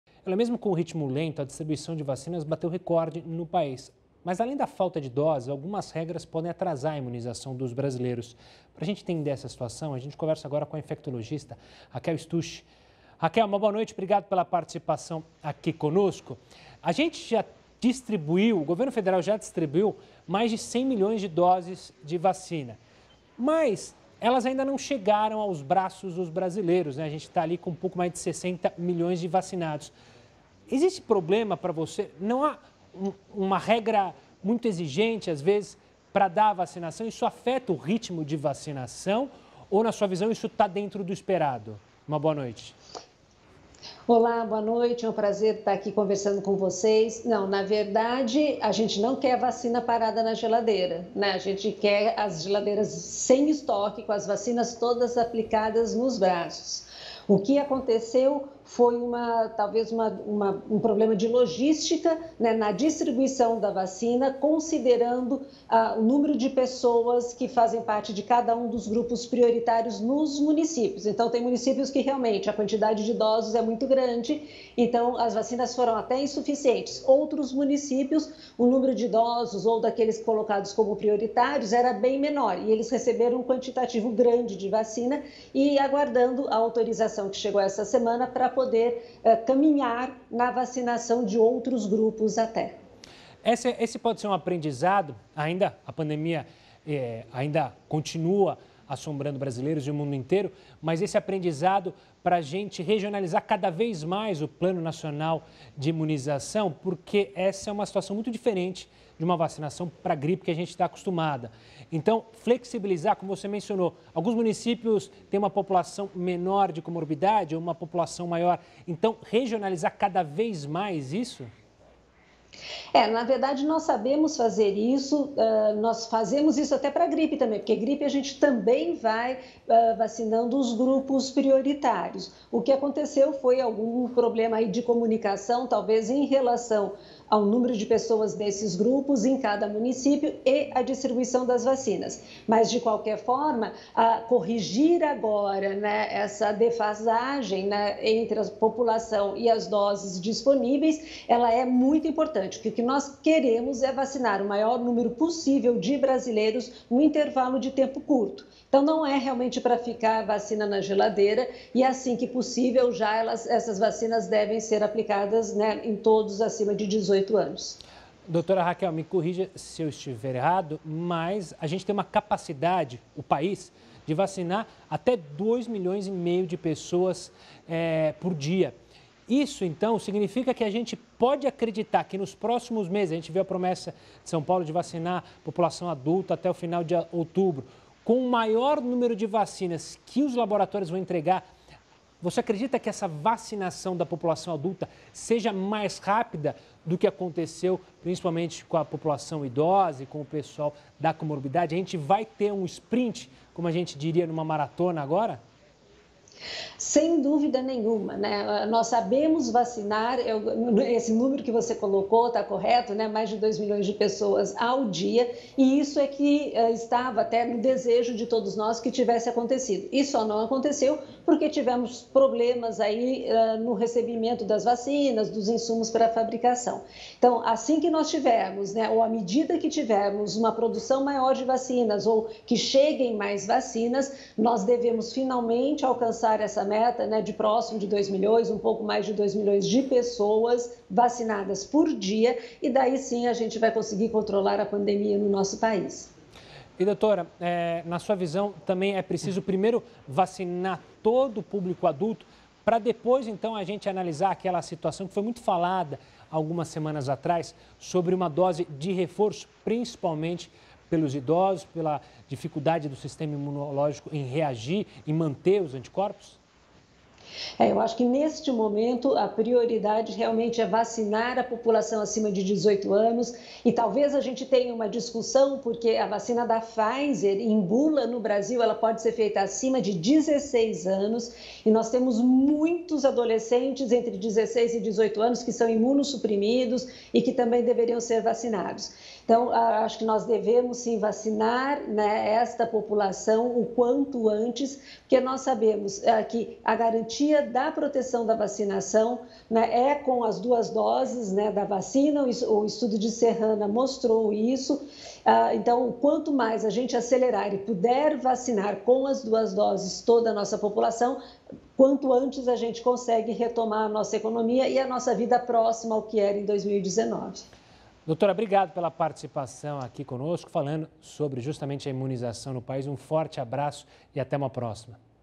Flexibilização de regras pode acelerar vacinação no Brasil
Mesmo com o ritmo lento, a distribuição de vacinas bateu recorde no Brasil no mês de maio. Mas além da falta de doses, algumas regras podem atrasar a imunização dos brasileiros. Para entender essa situação, conversamos  com a infectologista Raquel Stucchi.